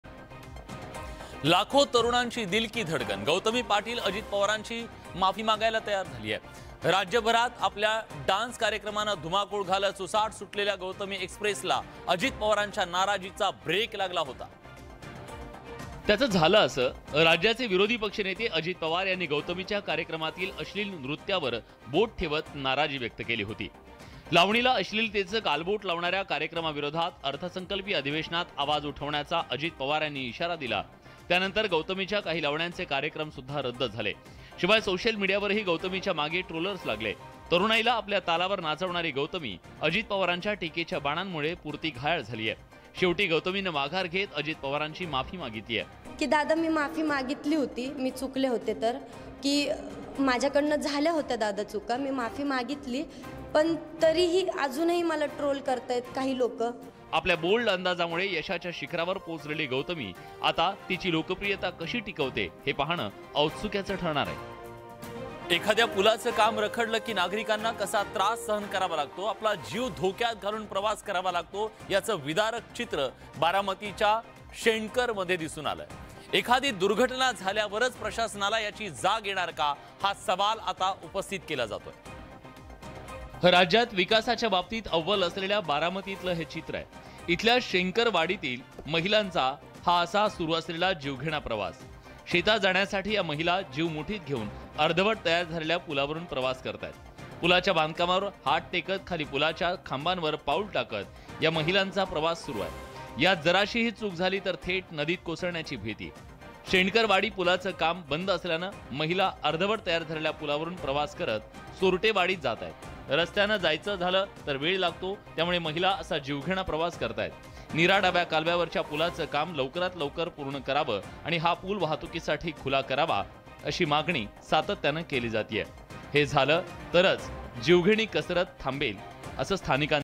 Gautami Patil on Ajit Pawar: अजितदादांचा नाराजीनामा, गौतमीचा माफीनामा | NCP | Maharashtra Politics
Gautami Patil is currently being heavily discussed in the state. However, she was always in the limelight due to her controversial gesture. On this, Ajit Pawar had said that he would raise this question in the session. On this, Gautami Patil has apologized to Leader of Opposition Ajit Pawar saying that I made a mistake earlier and apologized for it repeatedly, now I will not repeat that mistake, Dada forgive me.

राज्यात सध्या गौतमी पाटीलची जोरदार चर्चा सुरू आहे. मात्र, तिच्या वादग्रस्त हावभावामुळे ती नेहमी चर्चेत आली होती. यावर अजित पवारांनी हा प्रश्न अधिवेशनात उपस्थित करणार असे म्हटले होते. यावर गौतमी पाटील हिने मी पूर्वी चुकले आणि त्यासाठी वारंवार माफी मागितली, आता ती चूक मी परत करणार नाही, दादा मला माफ करा, असे म्हणत हात जोडत विरोधी पक्षनेते अजित पवार यांची माफी मागितली आहे.

n18oc_news